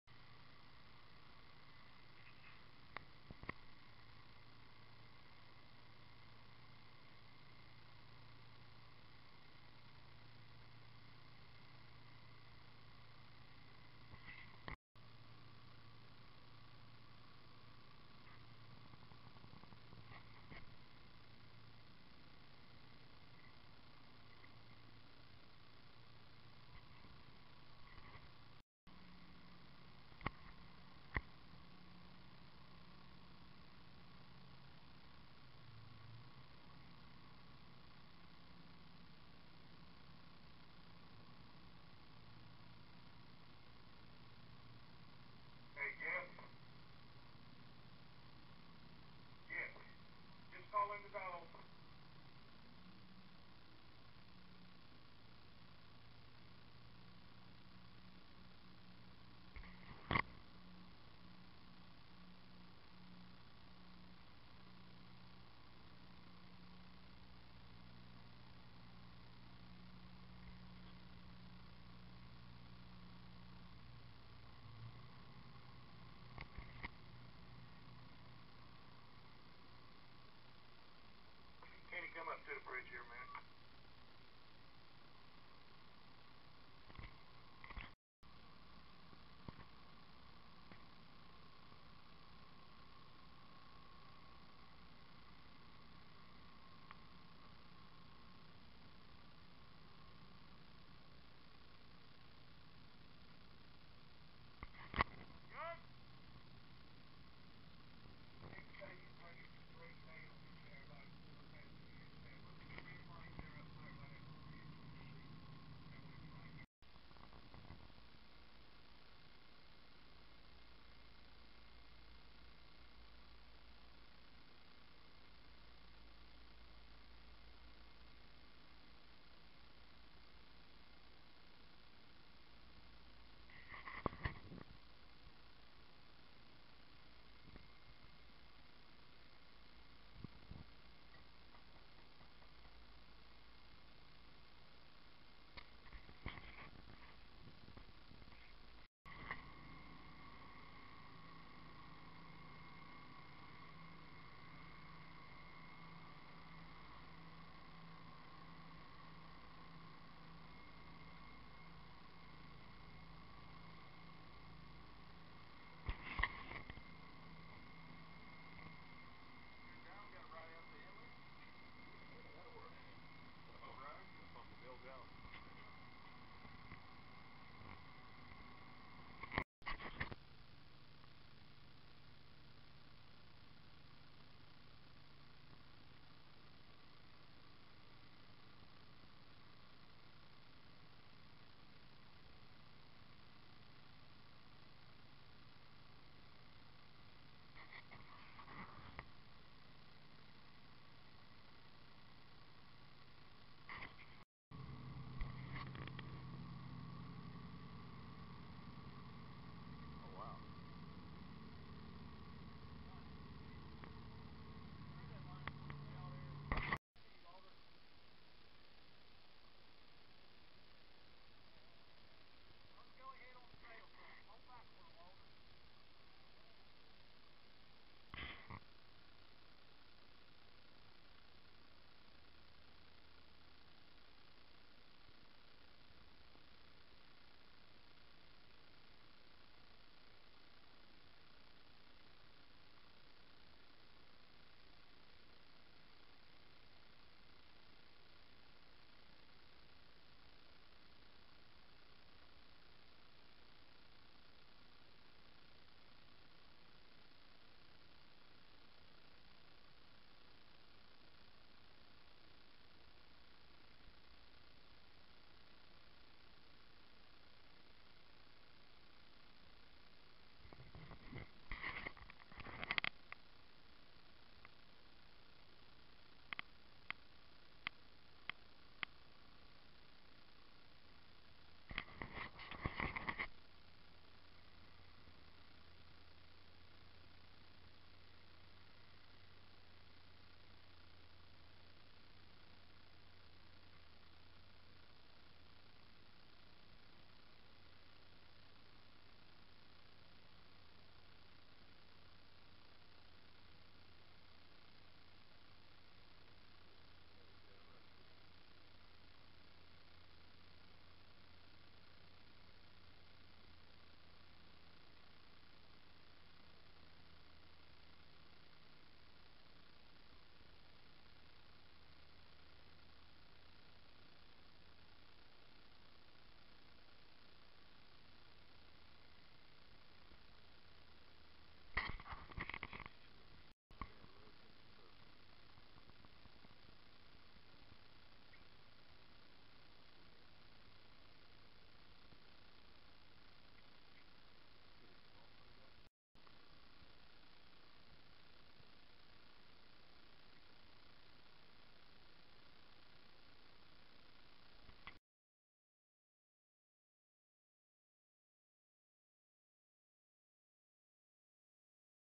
Carolina Princess Prepares for the upcoming Fishing Season!
Cleaning and repainting the bottom for the new season! Filmed using a Vivitar DVR 785HD